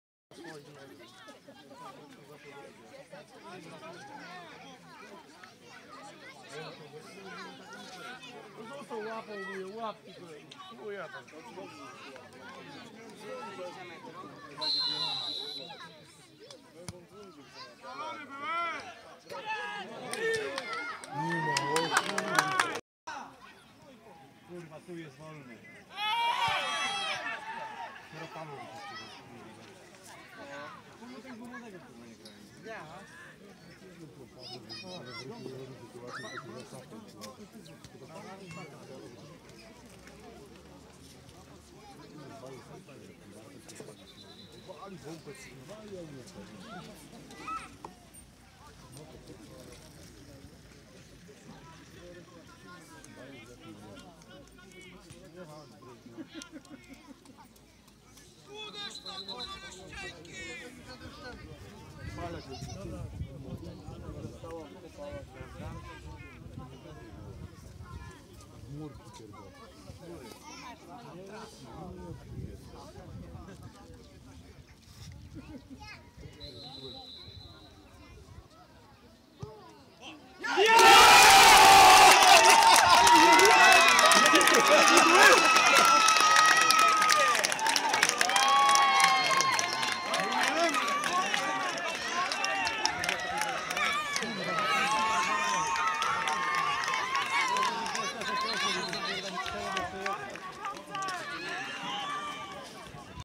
Start Niechcice vs Astoria Szczerców (1:1) 2:1 - Awans do okręgówki
Ostatnia kolejka, wywalczony awans Startu Niechcice do ligi okręgowej. Na filmie pięknej urody bramka dająca prowadzenie startowi, strzelcem B. Rakowiecki